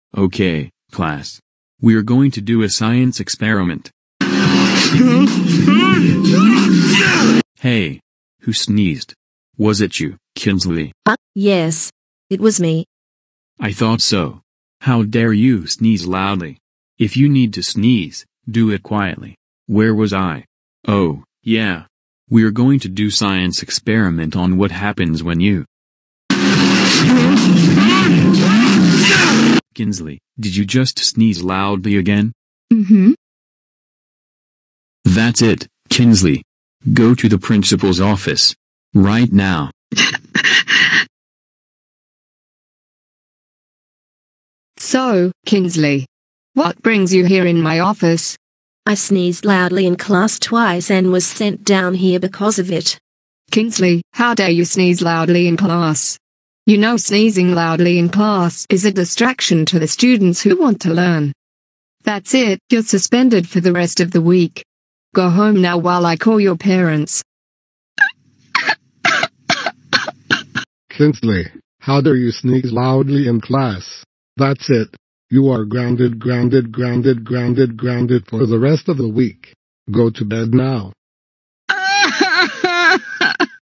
Kinsley Sneezes Loudly In Class And Gets Grounded
Inspired by Jacob Wright.